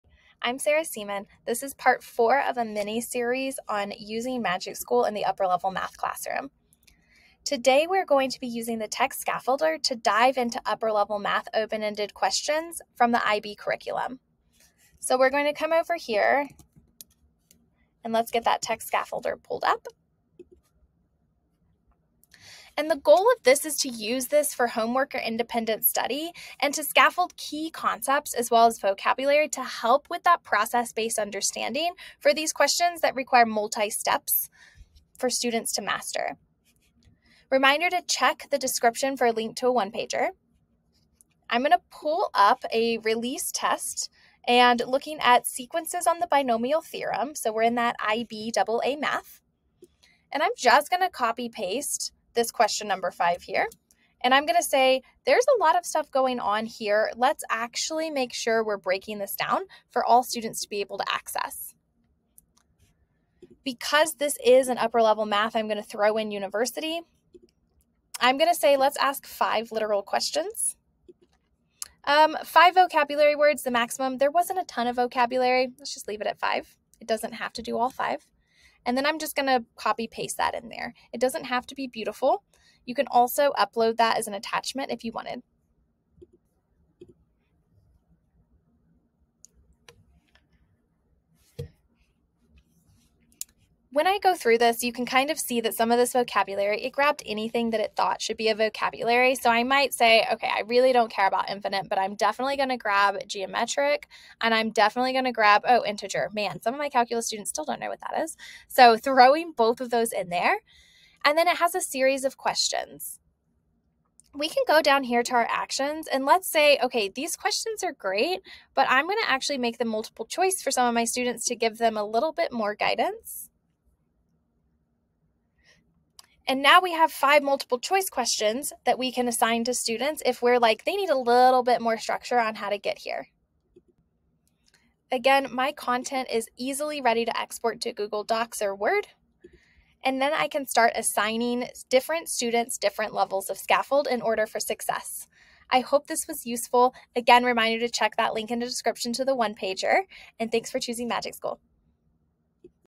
Pt. 4 Text Scaffolder
Using the Text Scaffolder to level and differentiate key topics in the secondary math classroom. Specifically using an IB Math AA topic, Sequences and Binomial Theorem from a multipart open-ended question.